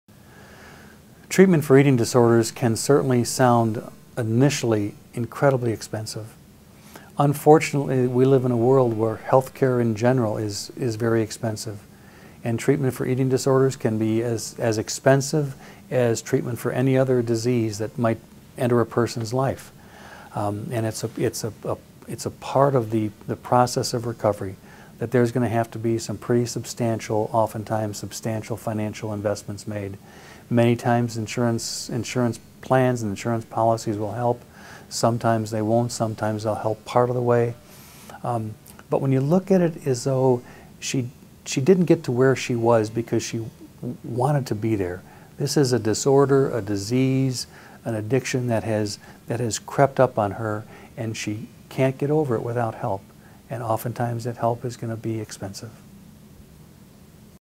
Why are Treatment Centers so Expensive?
"Why is treatment for eating disorders so expensive? Is it really worth all that money?"

Answered by Ward Keller, Founder, Remuda Ranch.